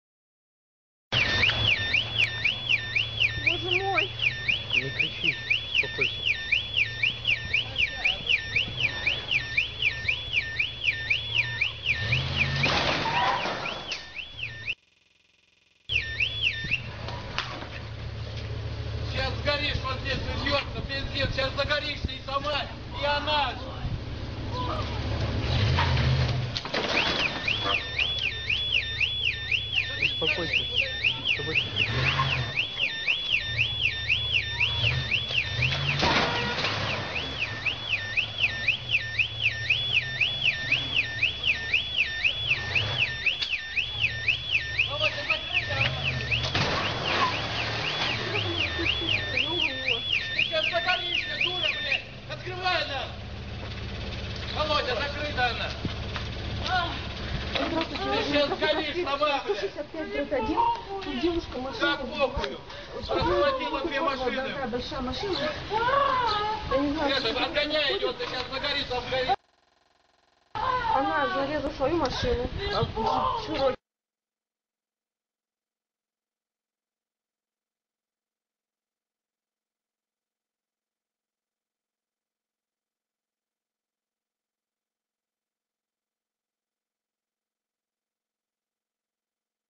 boom effects cars hidden funny fun
War of Neighbours

As you shouldn't do if you don't want to lose life

As it isn't necessary to divide a parking space that neighbors do what to occupy the niche.

3D Why not  to take the wheel in a state of intoxication.

Actually the Wives don't need to change, here that can occur.

The tire Very much loves the husband on it showed him that you shouldn't afflict the weaker sex! and That it is possible to lose the man's advantage!

Unfaithful spouses are waited by the same to consider as this poor creature!

Isn't necessary was to change to the wife! I And KNEW!

Probably not to turn out to tell any more that: At us with your girlfriend there was nothing not! All this is simple Rozygrysh! And the Brassiere is a gift to you on our Anniversary!

Why you don't trust me!  funny fun